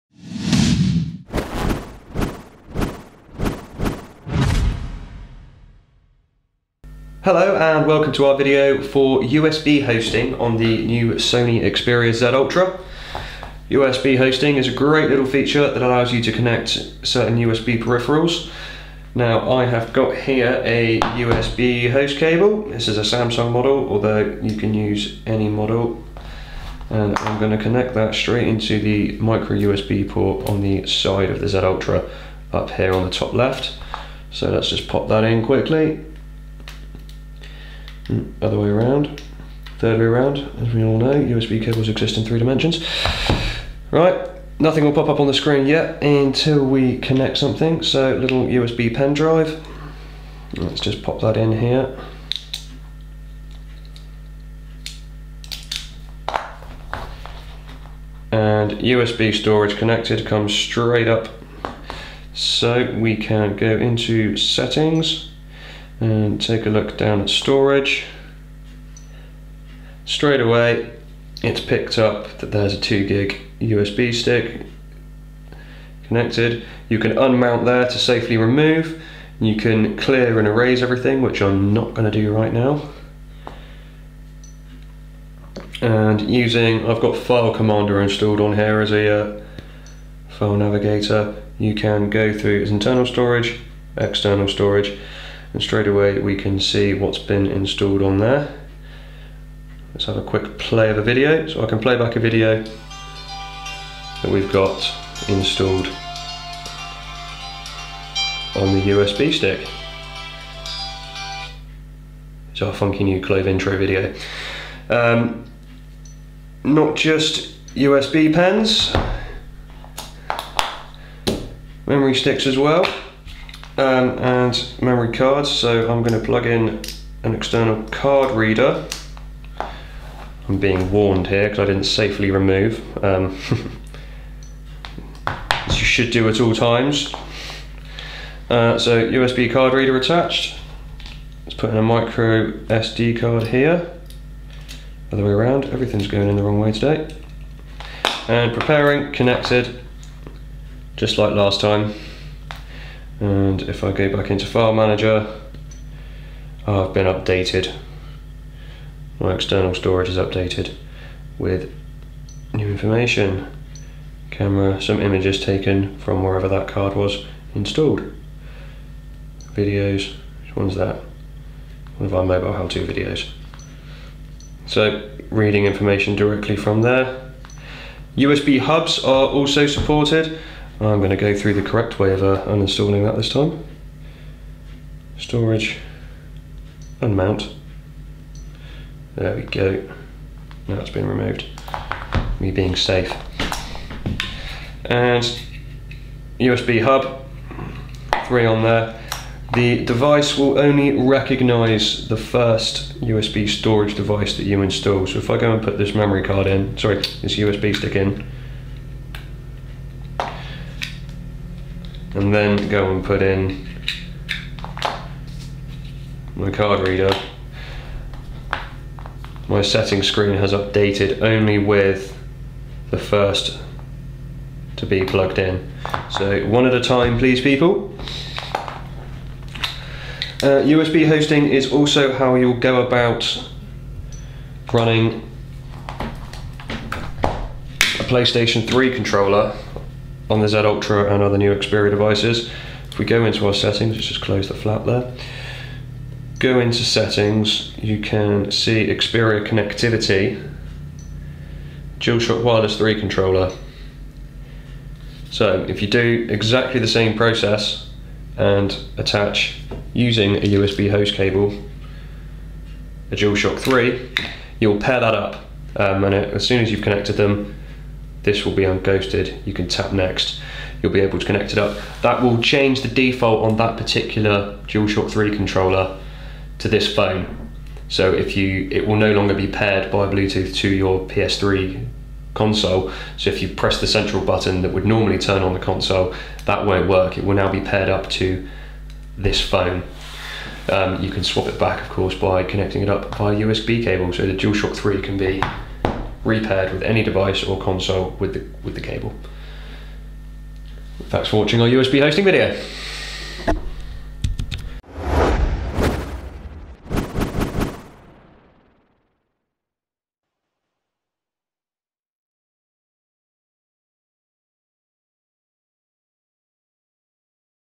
Sony Xperia Z Ultra USB Hosting/OTG Demo
In this video we show you how you can connect mass storage devices like USB memory sticks and memory cards to the Sony Xperia Z Ultra.